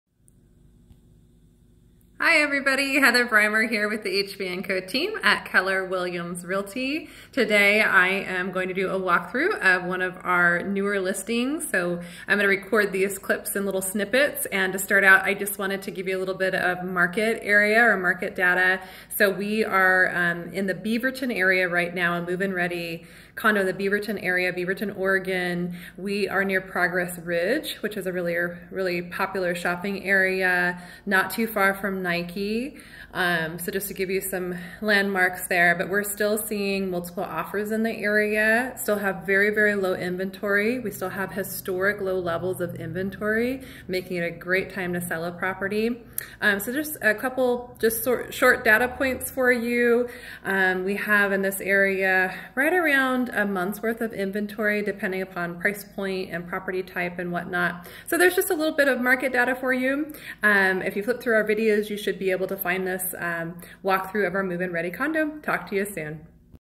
Beaverton, Oregon Real Estate Market
Market data for Beaverton, Oregon real estate market. Reach out with your market questions.

Heather Brymer, HB & CO. Team
Oregon Principal Real Estate Broker
Washington Real Estate Broker
W: HBandCORealtyGroup.com